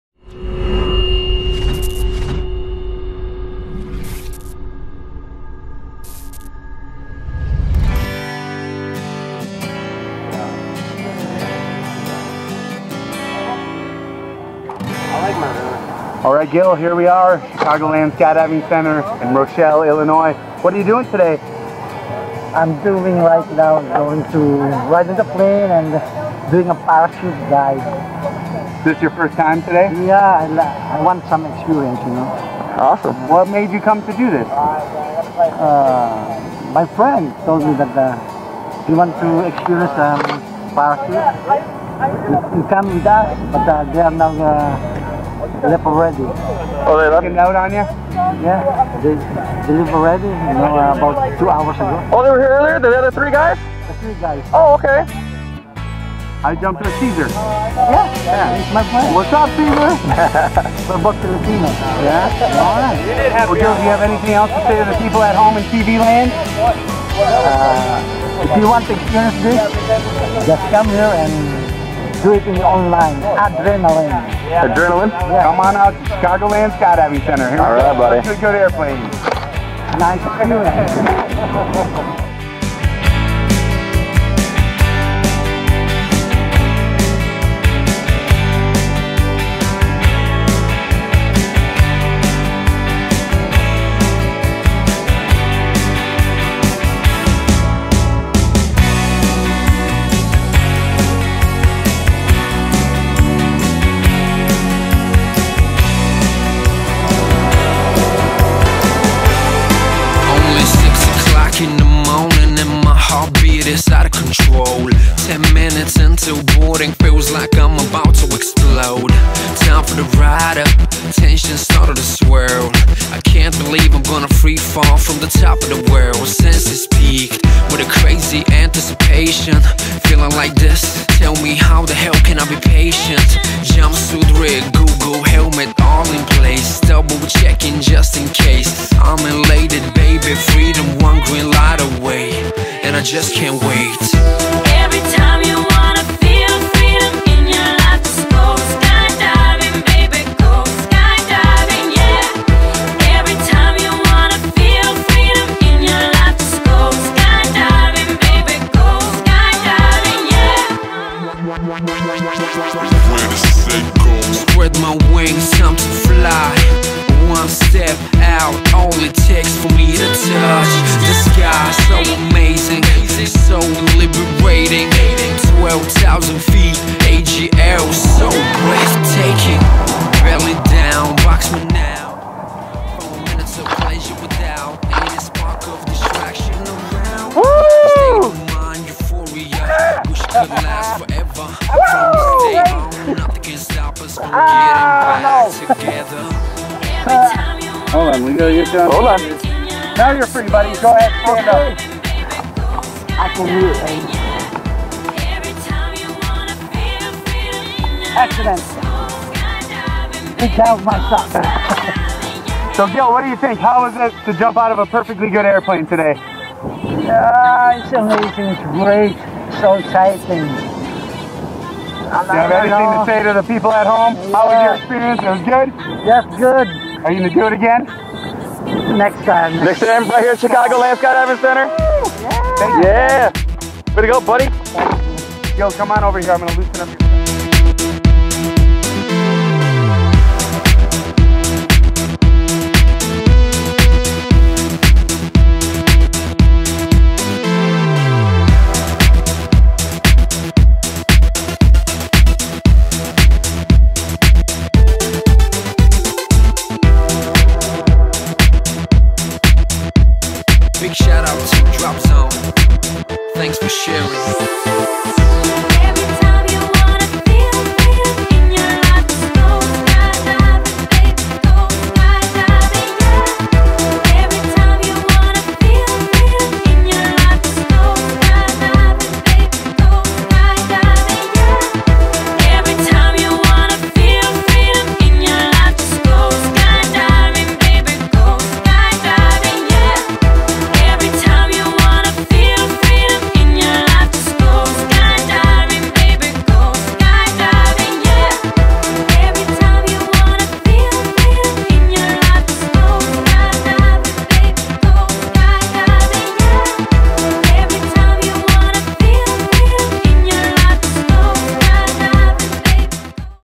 Gil at Chicagoland Skydiving Center
Gil came out to jump with us only hours after his friend Ceasar had returned home and shared his tandem video from that morning.  If you are thinking about skydiving, don't put it off, the time is now.